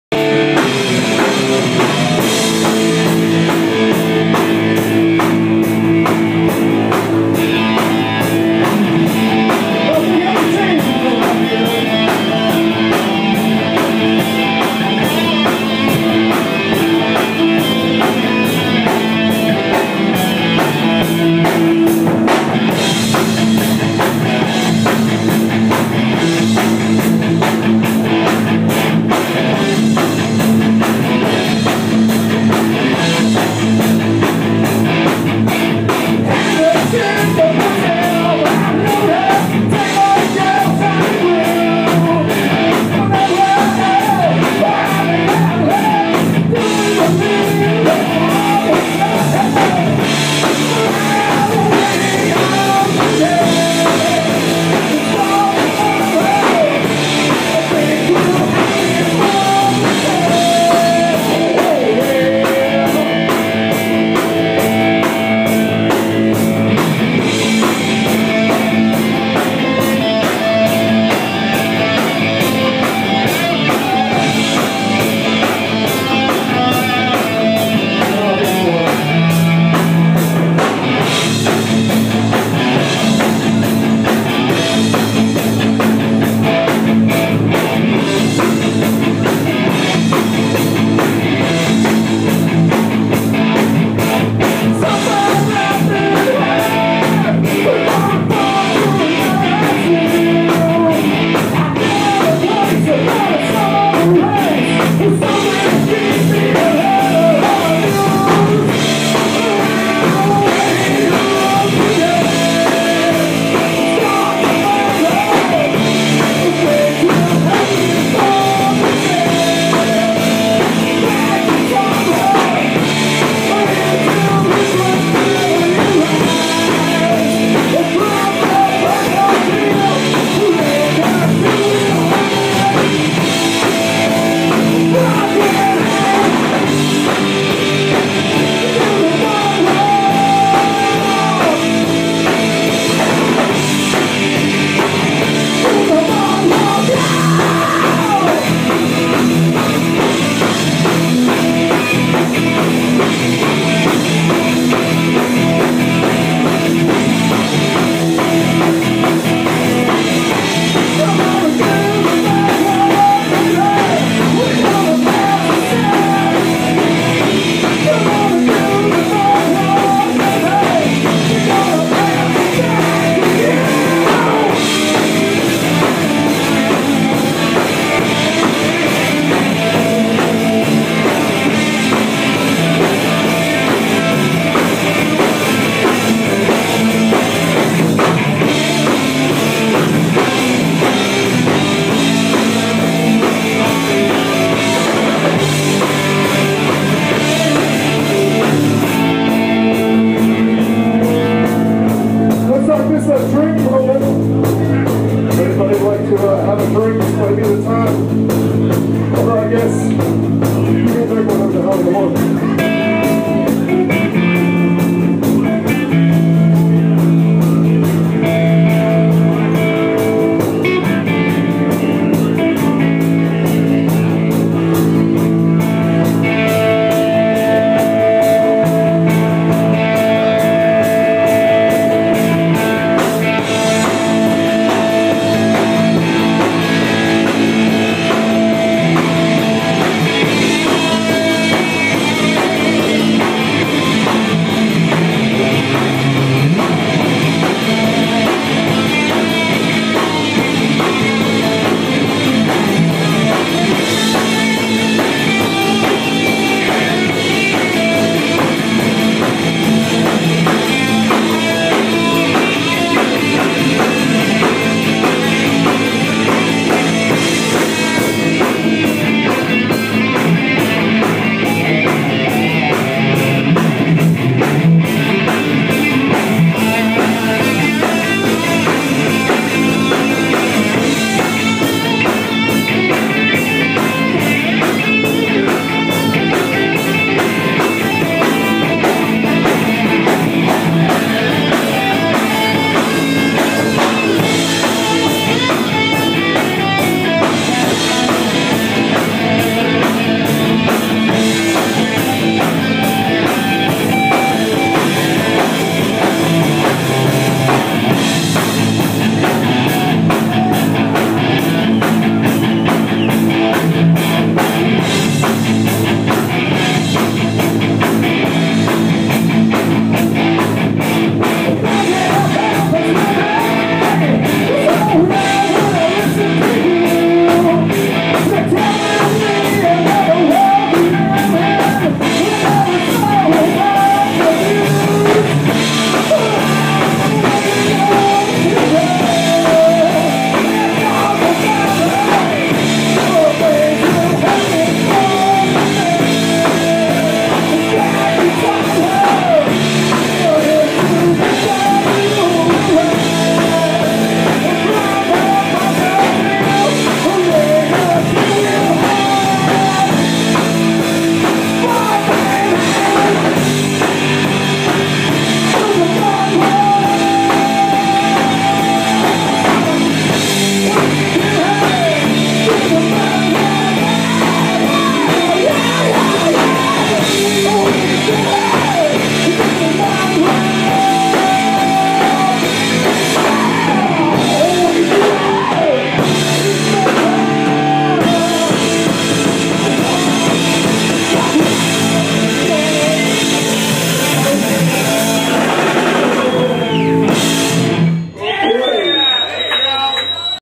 The diePods * Jimmy's Saloon * 11.23.13
Newport's own, The diePods, rock the crowd at Jimmy's Saloon on Saturday, November 23rd as part of the diePods and Neutral Nation show.